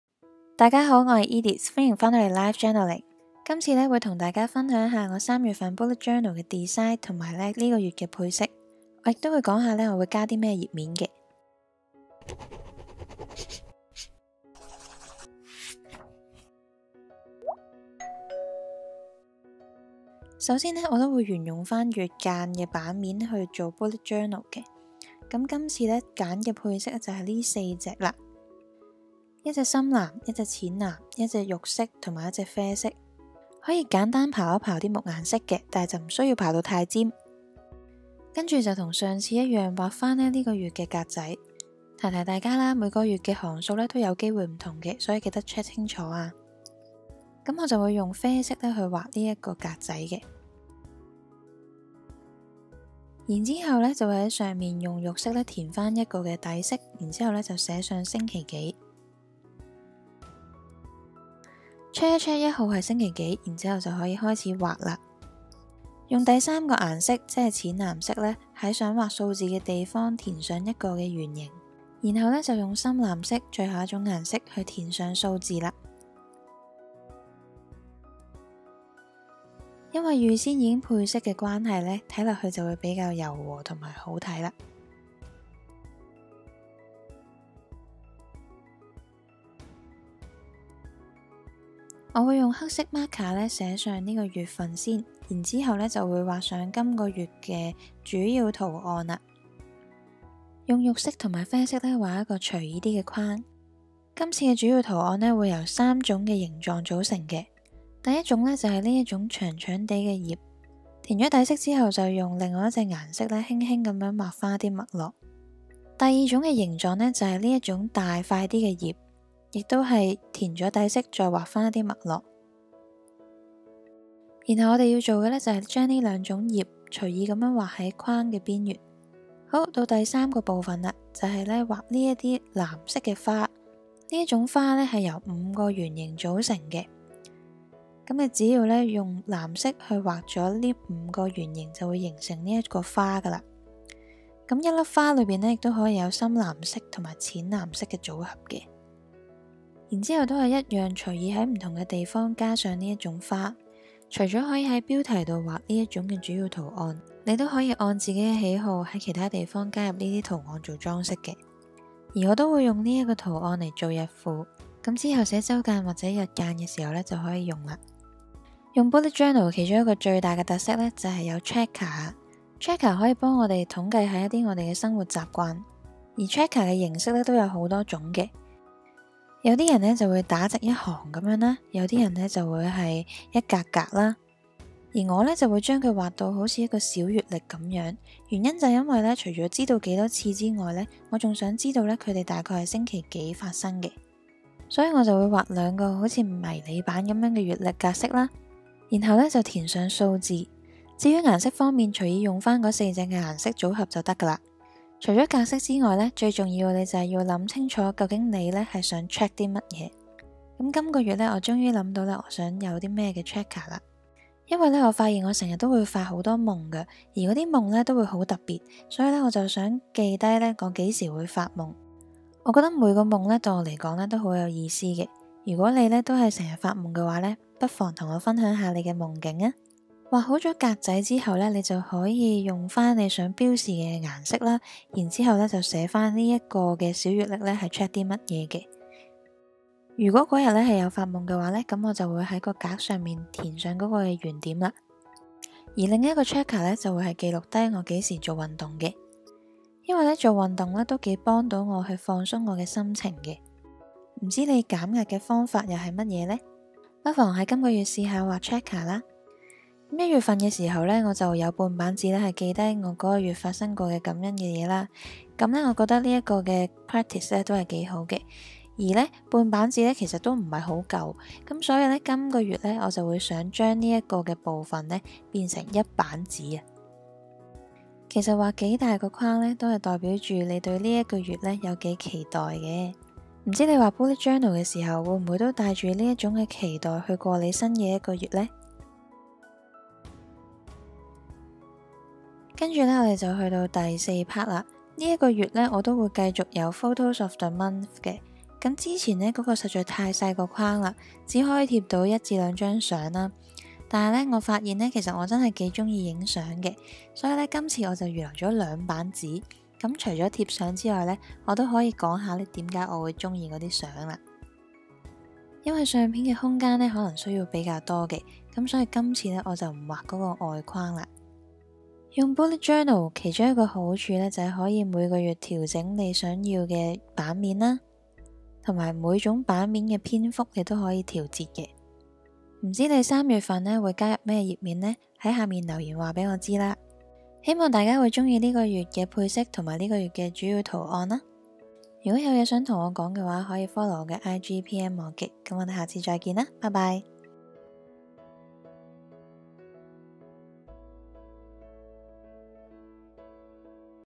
March 2019 Bullet Journal ｜簡約風 每月配色｜ 三月手帳設計｜Edith
2019 年齊來寫手帳吧～
這系列的子彈筆記
會以每個月不同的配色
造出不同的風格

坊間有很多很精彩的Bullet Journal
每個人喜歡的風格也不一樣
我喜歡的是比較簡單的
花在畫的時間也會較少
真正在寫手帳的時間也會相對多一點